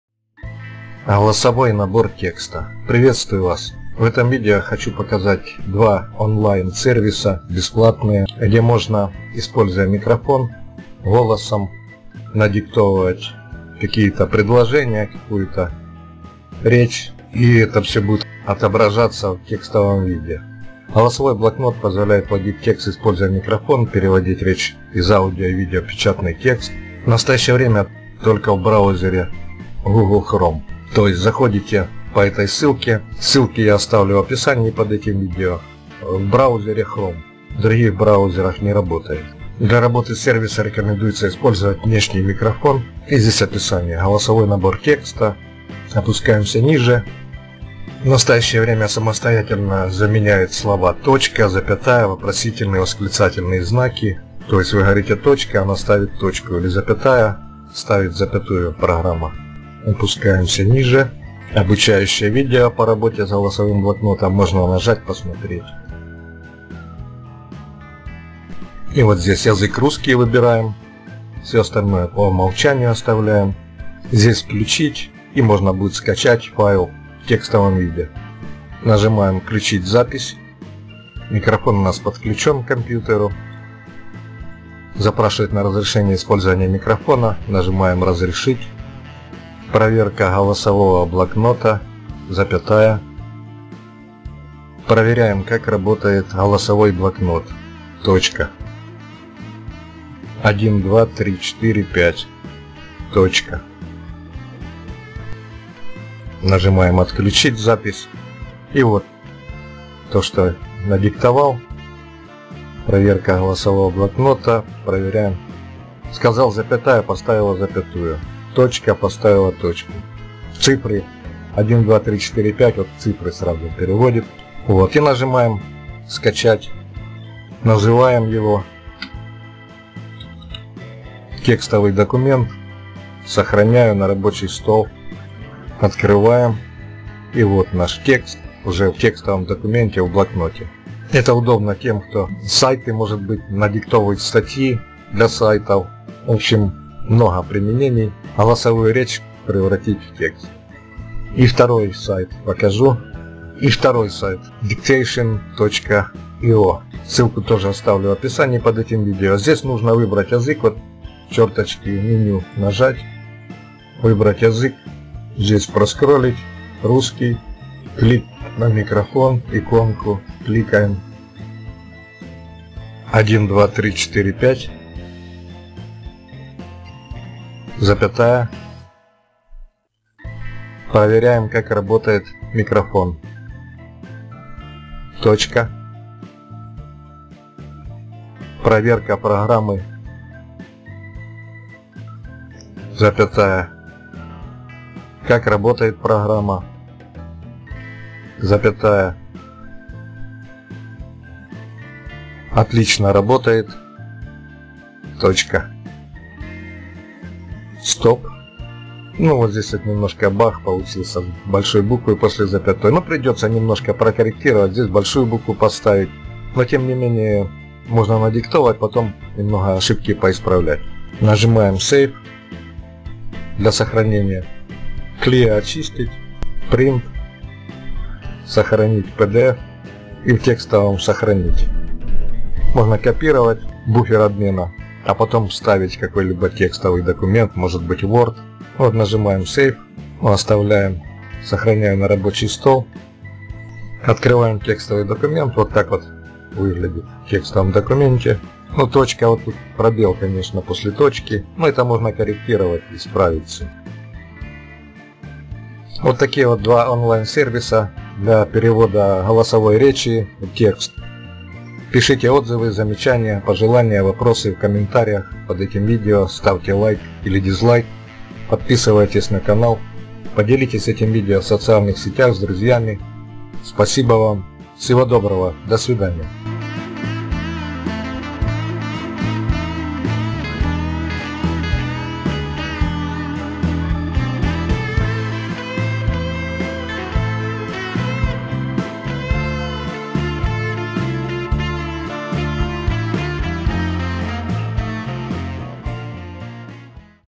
Перевод речи в текст. Голосовой набор текста
Перевод речи в текст. Голосовой набор текста это бесплатный онлайн сервис, который позволяет переводить речь в текст, используя микрофон, и сохранять этот текст в текстовый документ.